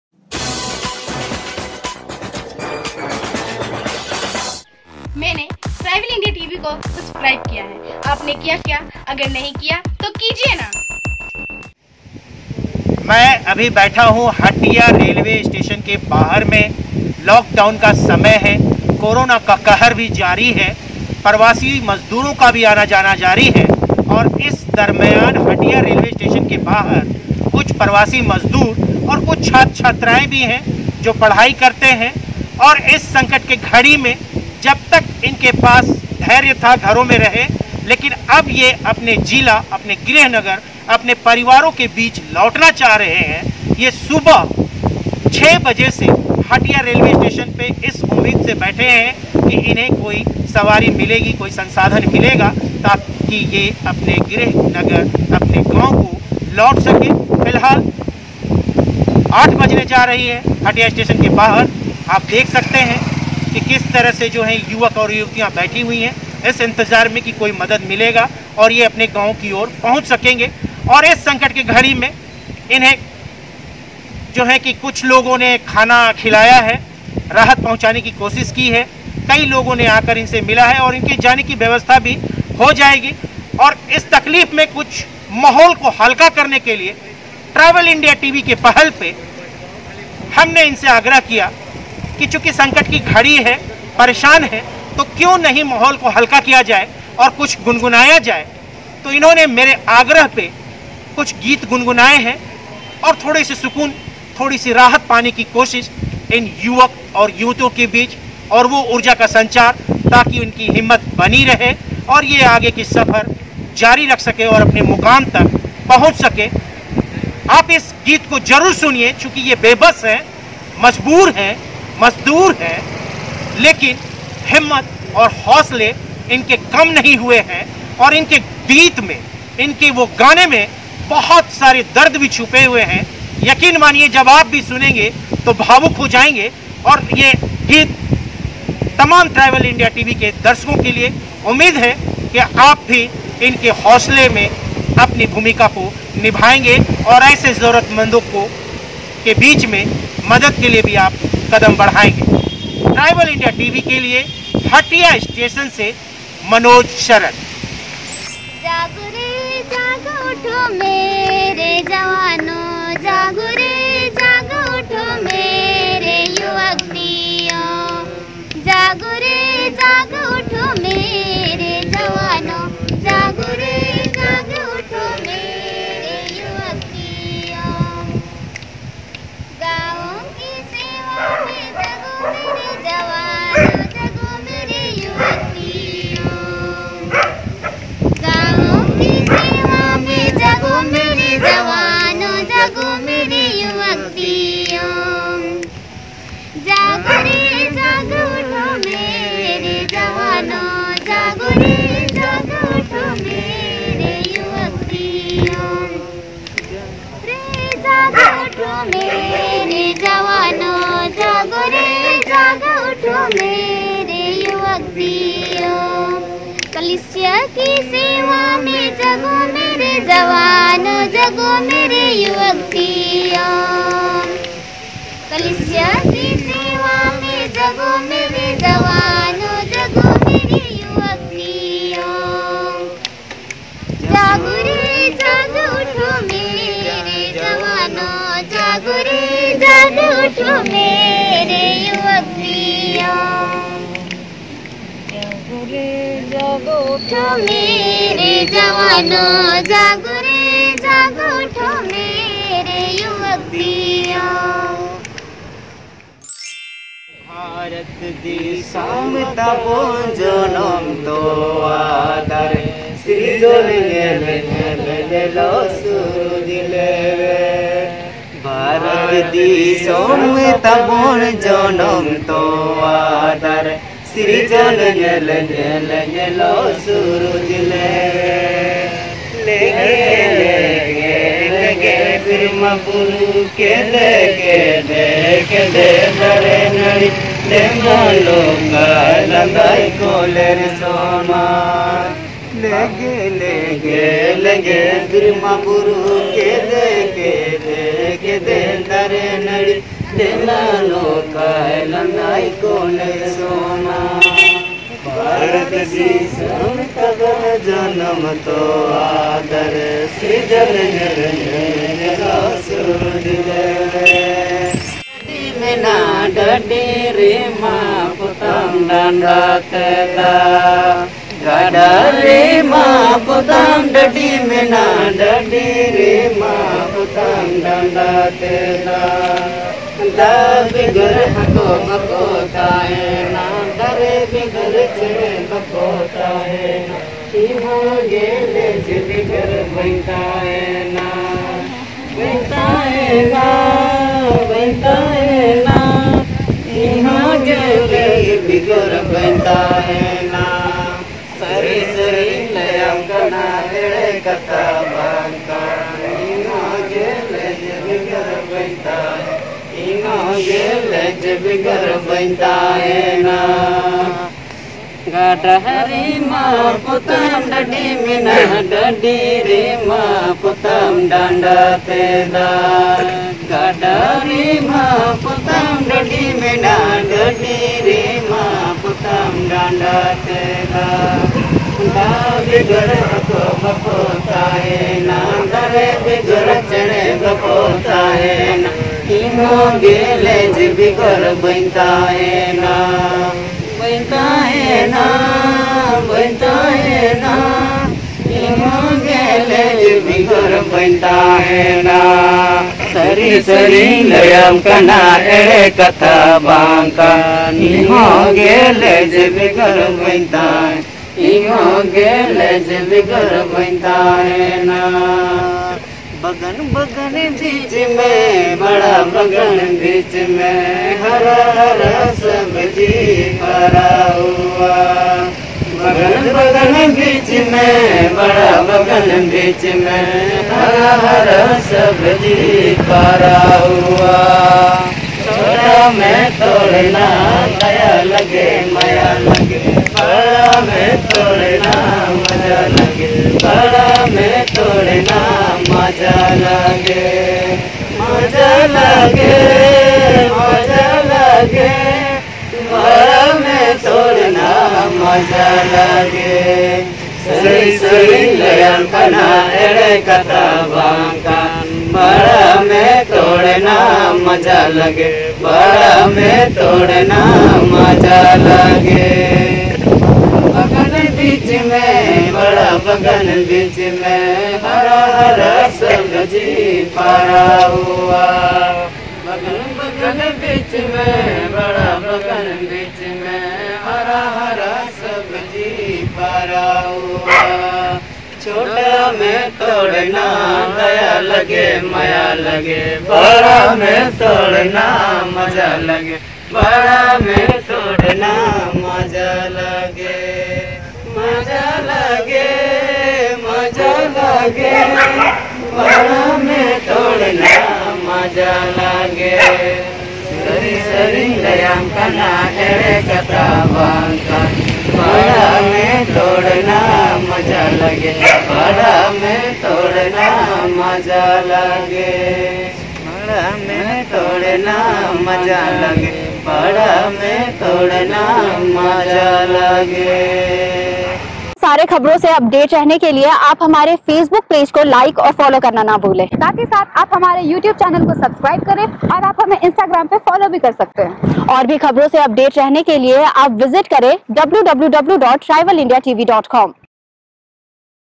Jharkhand : प्रवासियों की दर्द भरी आवाज सुनकर हो जाएंगे भावुक || santhali song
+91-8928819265

Jharkhand Bureau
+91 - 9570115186